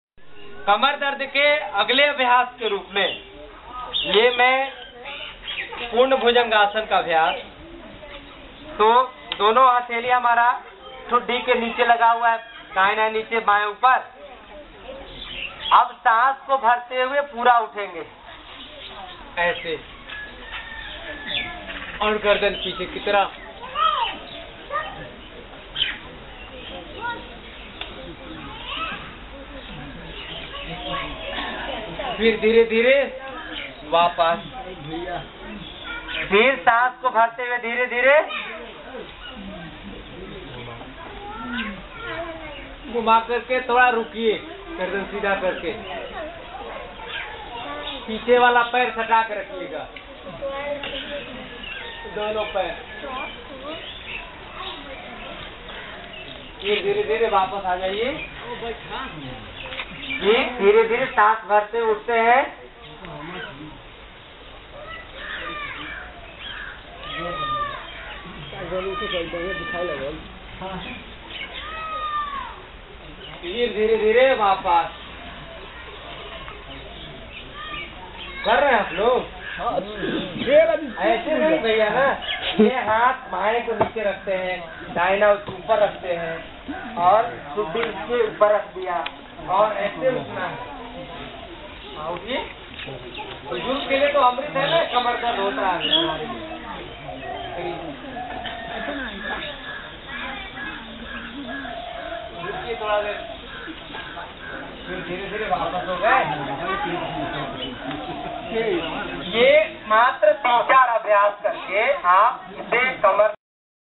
Yog Shivir|Yogi Dr.Brij Mohan |Yog Pracharak-Patanjali Yogpeeth,in Lalganj-Garhani-Bhojpur-Bihar
02 State-BIHAR,District-BHOJPUR,Month-September-2016,Camp No.-02,Time-05.00AM TO 08.30AM,Yog Shivir-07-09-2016 To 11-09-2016,Venue-Vill-Lalganj,Baligaon,Block-Garhani,District-Ara(Bhojpur),State-Bihar,Video Cliping  Date-09/09/2016